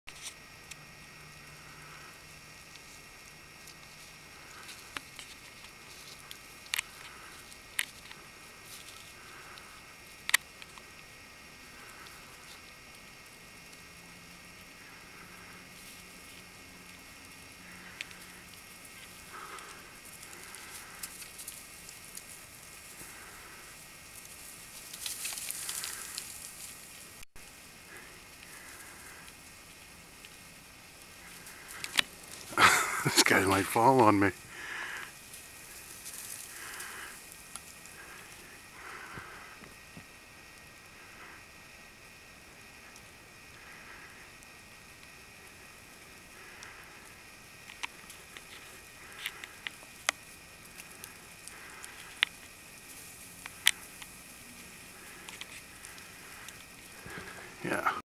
Porky In A Hemlock 2
The second of 2 videos of the porcupine who was hanging around last winter.  In this one he climbs to the top of the hemlock sapling, after which I decided to move back because they've been known to fall out of trees.